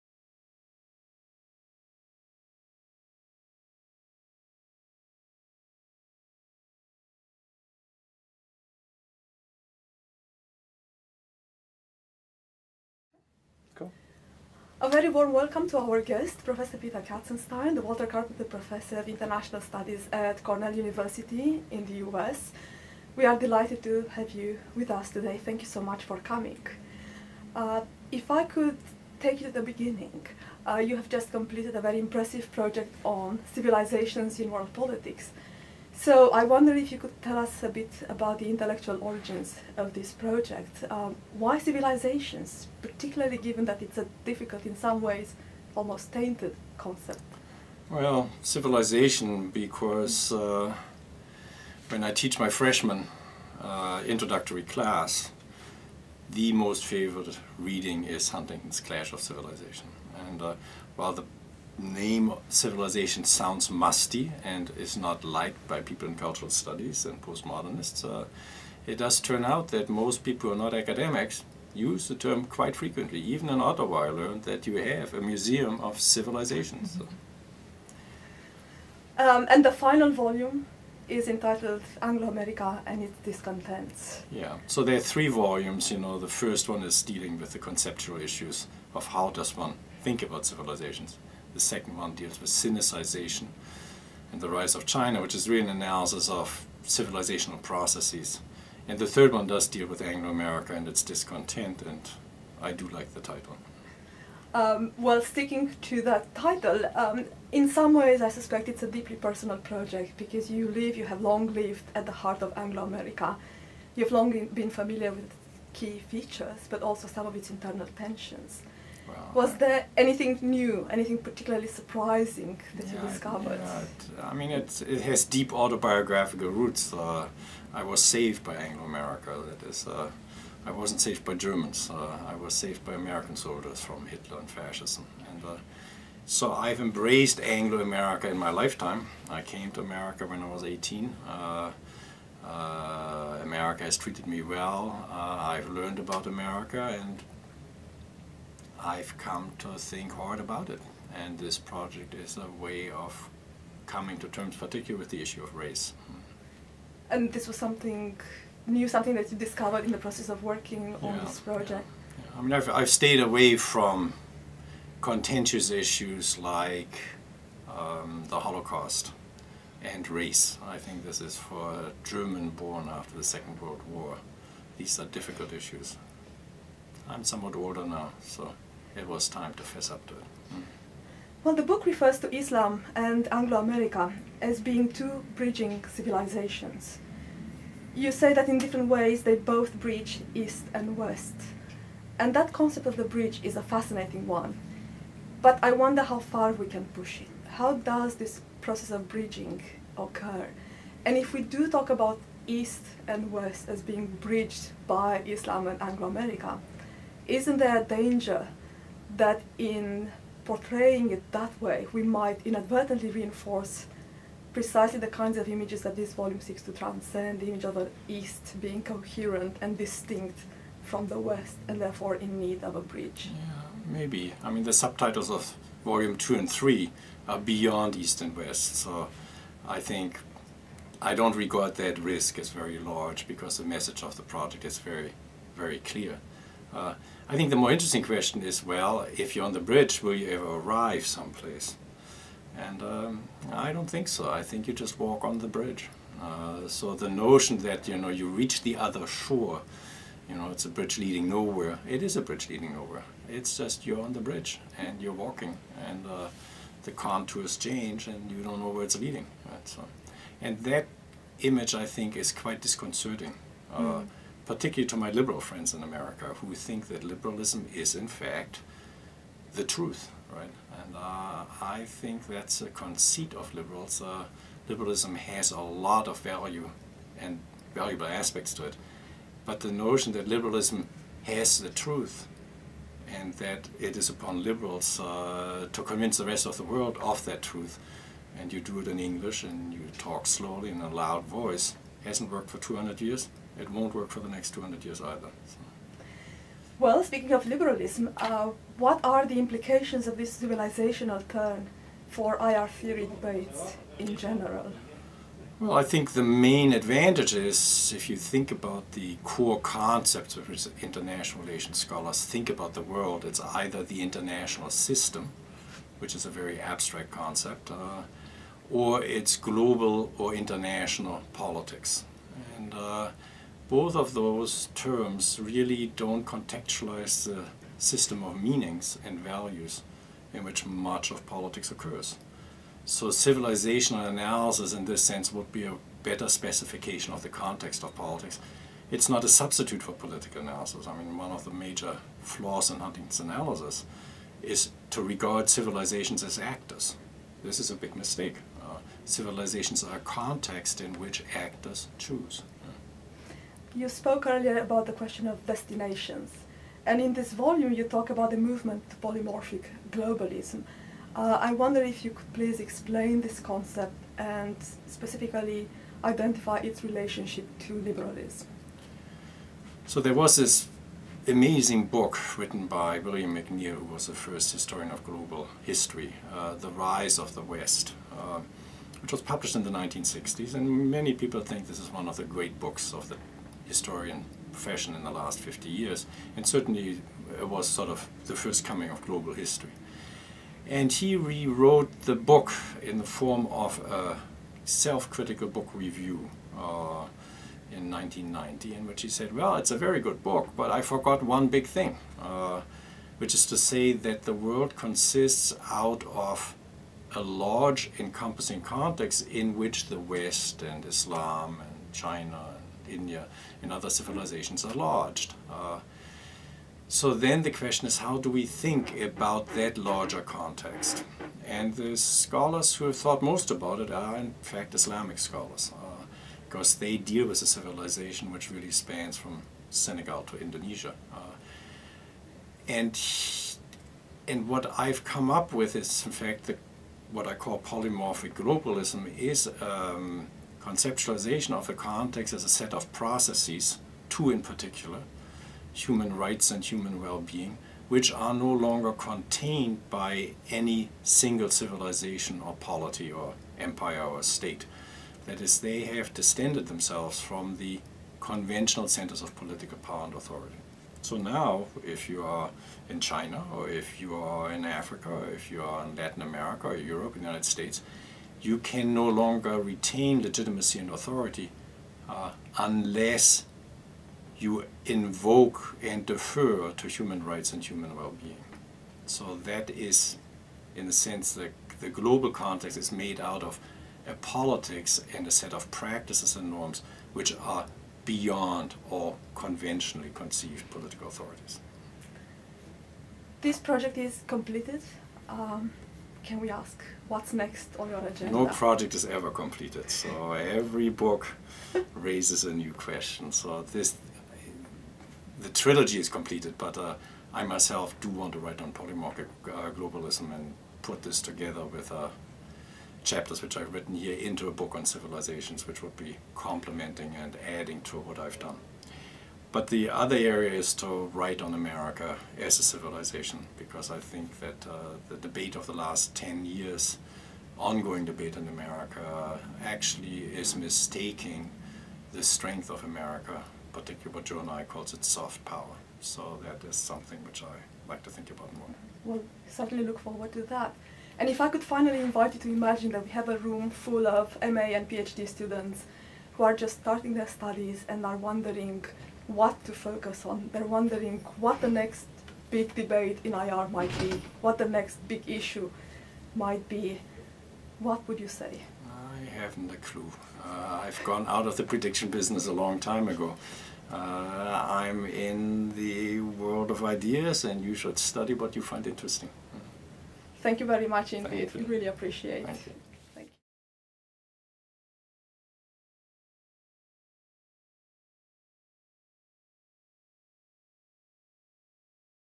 In conversation with Peter Katzenstein
University of Ottawa

Alexandra Gheciu, Associate Professor at the Graduate School of Public and International Affairs, spoke with Professor Katzenstein before his talk at CIPS on February 2nd, 2012.

Université d'Ottawa

Alexandra Gheciu, professeure agrégée à l'École supérieure d'affaires publiques et internationales, a discuté avec Professeur Katzenstein le jour de sa présentation au CÉPI le 2 février, 2012.